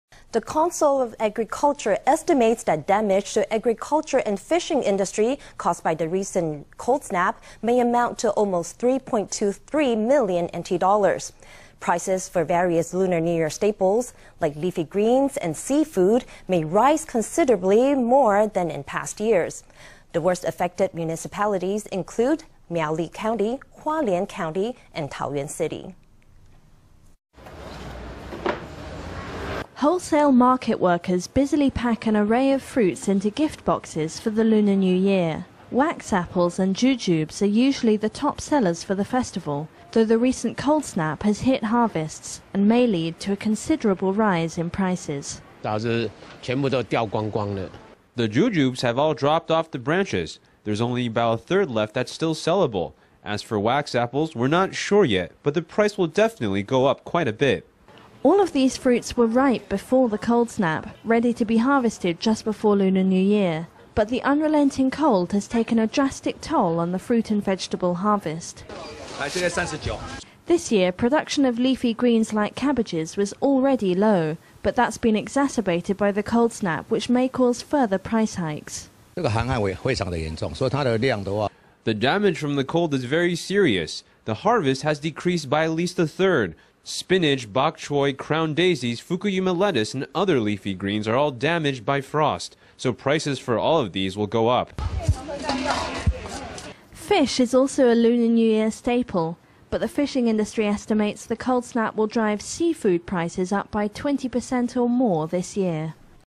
Cold snap reduces harvest of fruits and vegetables, driving up prices during holiday
The Council of Agriculture estimates that damage to agriculture and fishing industries caused by the recent cold snap may amount to almost NT$3.23 million. Prices for various Lunar New Year staples like leafy greens and seafood may rise considerably more than in past years. The worst-affected municipalities include Miaoli County, Hualien County, and Taoyuan City. Wholesale market workers busily pack an array of fruits into gift boxes for the Lunar New Year. Wax apples and jujubes are usually the top sellers for the festival, though the recent cold snap has hit harvests and may lead to a considerable rise in prices. Chen Tai-yuanFruit VendorThe jujubes have all dropped off the branches. There’s only about a third left that’s still sellable. As for wax apples, we’re not sure yet, but the price will definitely go up quite a bit.All of these fruits were ripe before the cold snap, ready to be harvested just before Lunar New Year. But the unrelenting cold has taken a drastic toll on the fruit and vegetable harvest. This year, production of leafy greens like cabbages was already low, but that’s been exacerbated by the cold snap which may cause further price hikes.Weng Jung-langVegetable VendorThe damage from the cold is very serious. The harvest has decreased by at least a third. Spinach, bok choy, crown daisies, Fukuyama lettuce and other leafy greens are all damaged by frost, so prices for all of these will go up.Fish is also a Lunar New Year staple, but the fishing industry estimates the cold snap will drive seafood prices up by 20% or more this year.